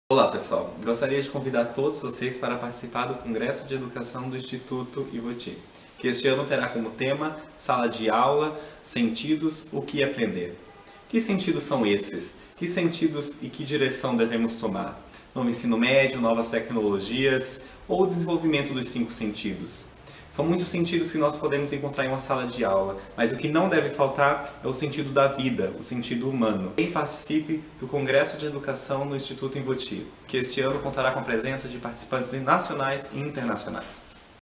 Diego Santana de Freitas | Acadêmico de Letras Português e Alemão
O acadêmico de Letras Português e Alemão, Diego Santana de Freitas, fez um convite especial para o congresso Sala de Aula e Sentidos: o que aprender? Que será dias 31 de agosto e 01 e 02 de setembro.